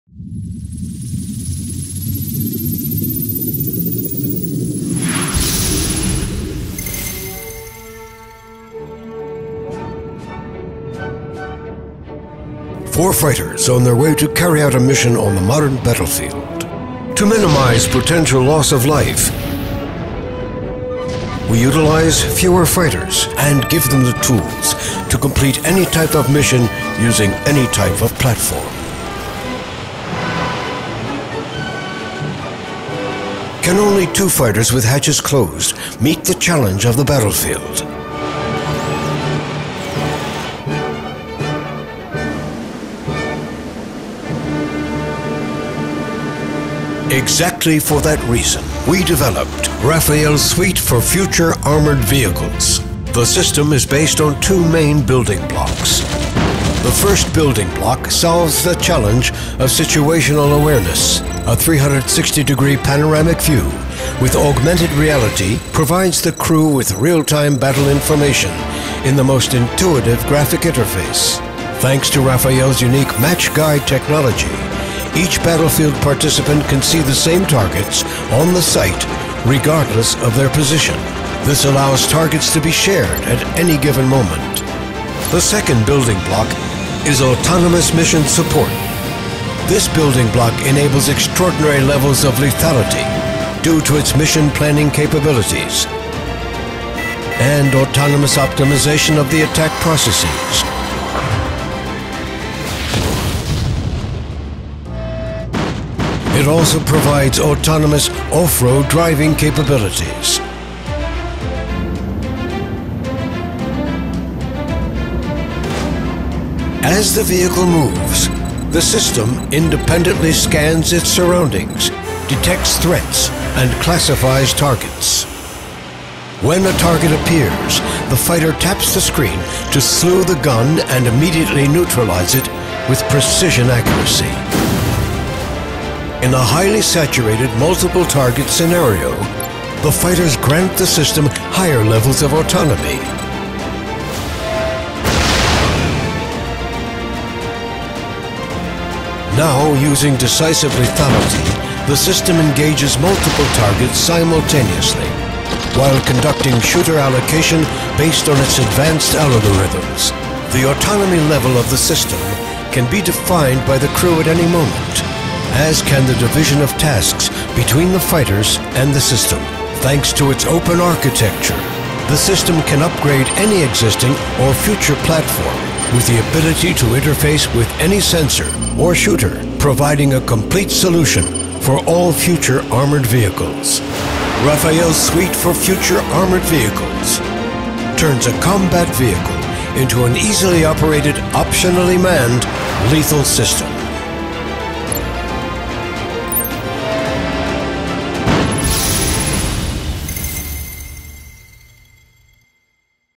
RAFAEL's Advanced Suite for AFVs - A Quantom Leap in Armored Vehicles Transformation
Integrating the most advanced technologies, capabilities & subsystems, RAFAEL's Suite for future armored vehicles
transforms any armored combat platform – including AFVs, APCs and tanks – into an ultra-modern, optionally manned connected, protected and lethal defense system with enhanced survivability and maneuverability.